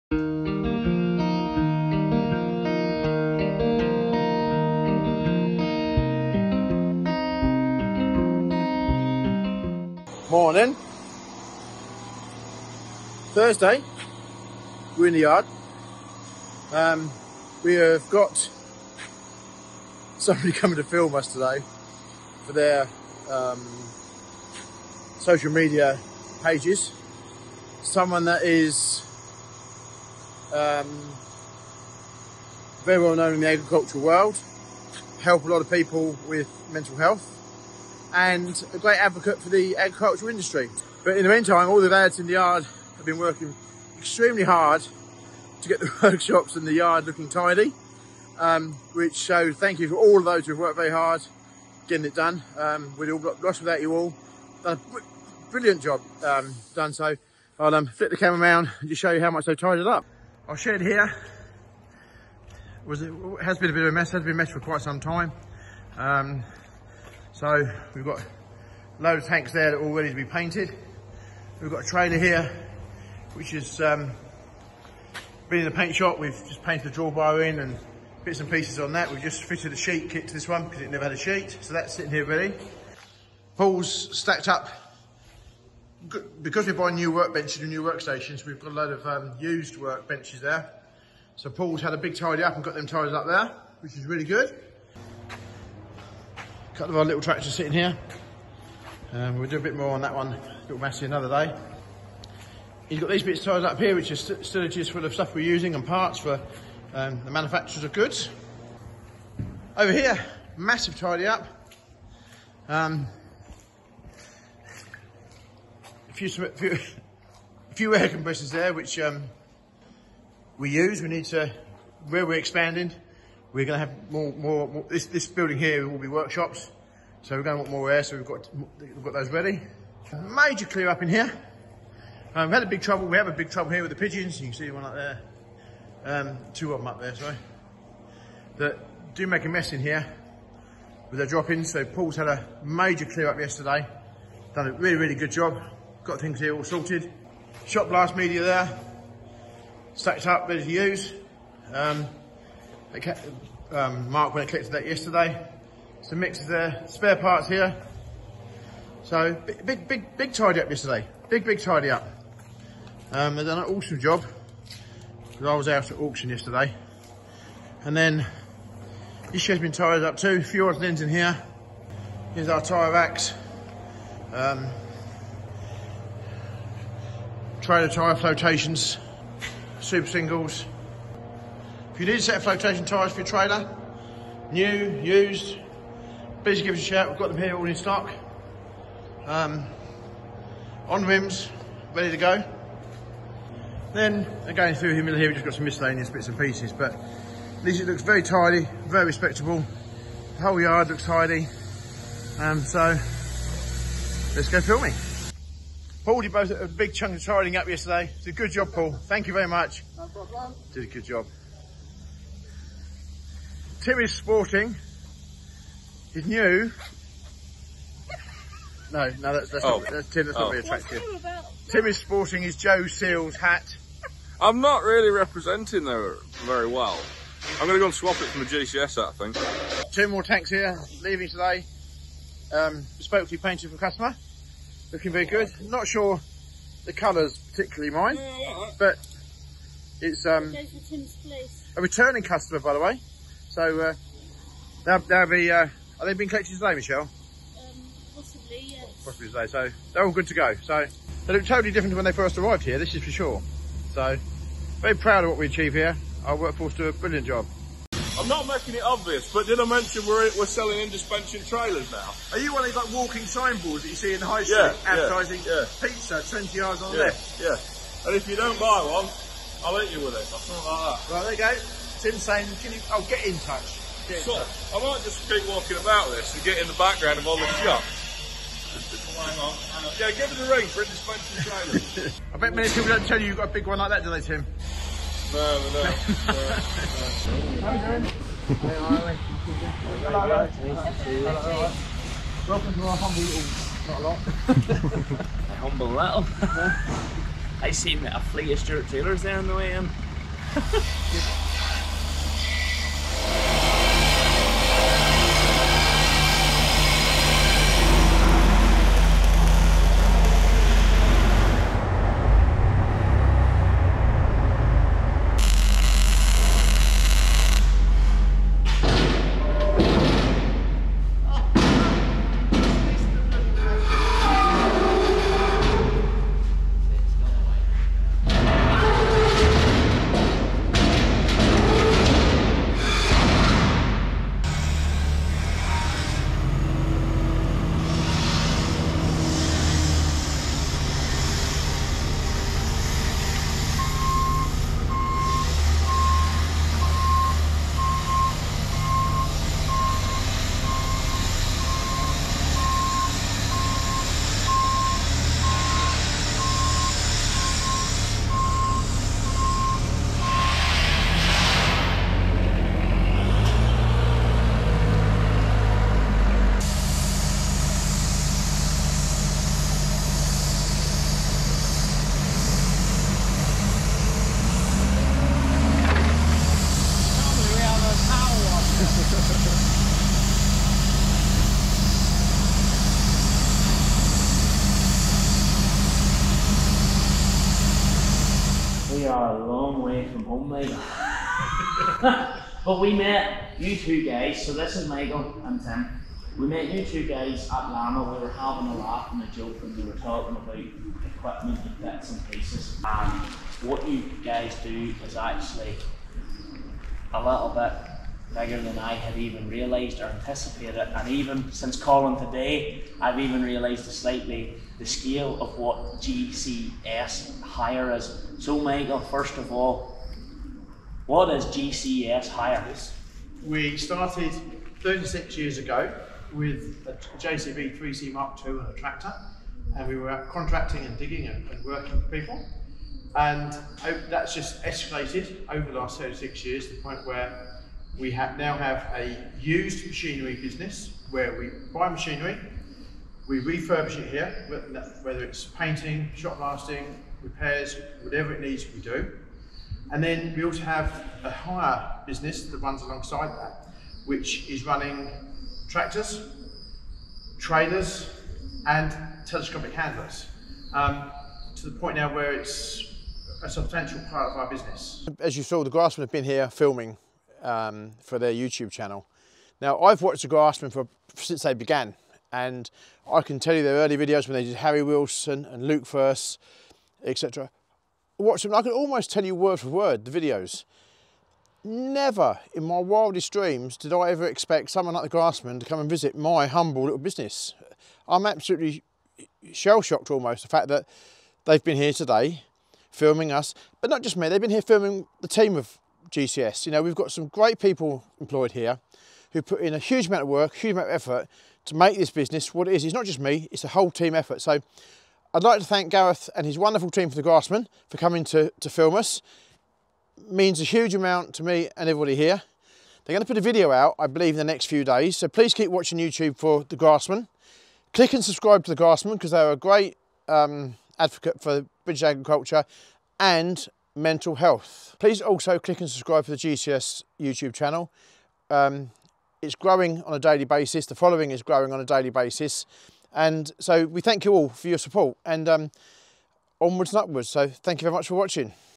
GRASSMEN visit the GCS Hire HQ!
Gareth and the @Grassmen team check out all things GCS Machinery, and Michael shows you what the team has done ahead of their visit.